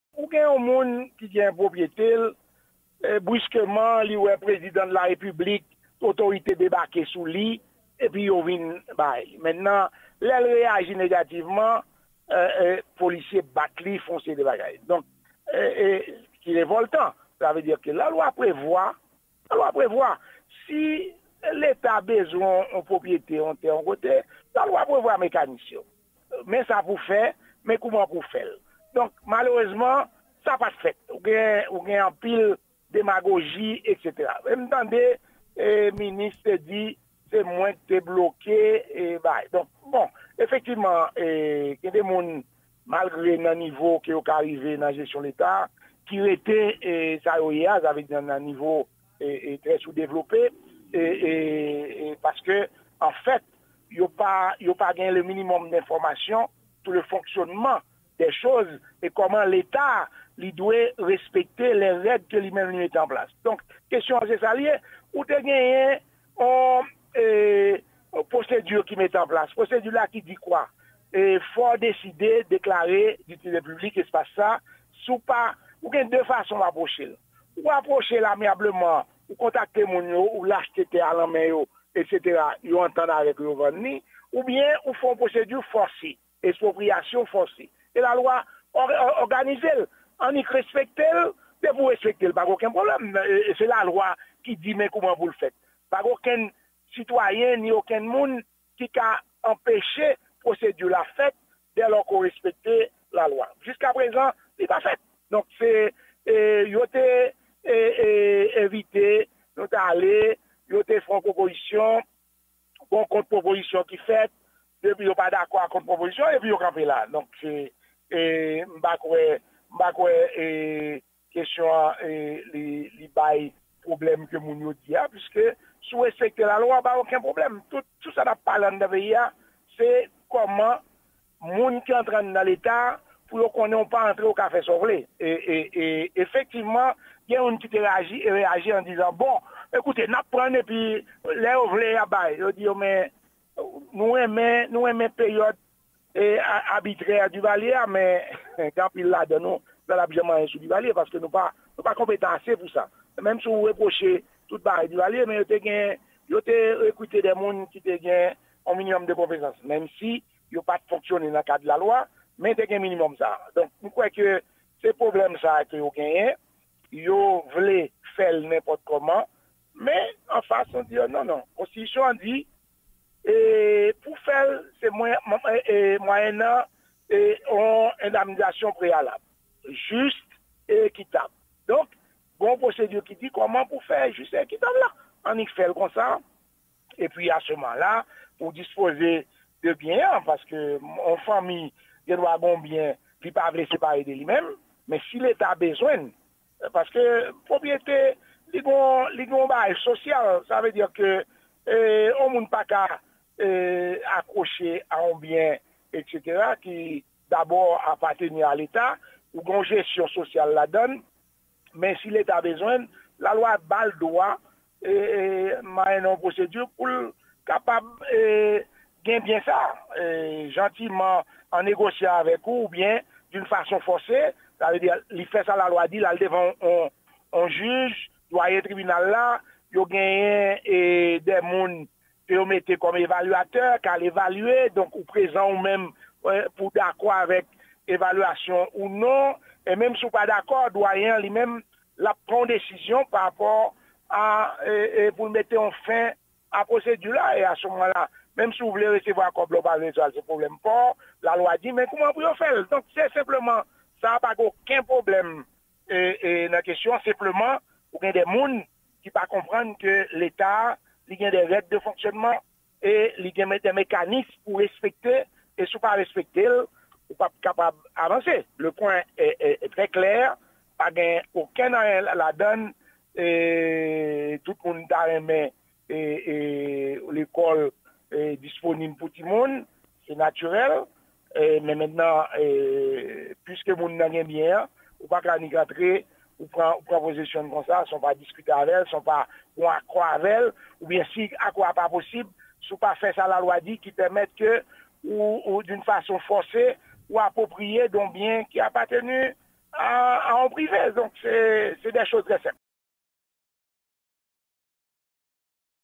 Me. Camille Leblanc / Dosye rekonstwiksyon Lise Jean-Jacques Dessalines
Mèt Camille Leblanc avoka pwopriyetè teren leta prevwa achte pou rekonstwi lise Jean-Jacques Dessalines nan fè konnen se pwosedi yo ki pat respekte ki lakoz leta Ayisyen pa achete espas pou konstwi lise a.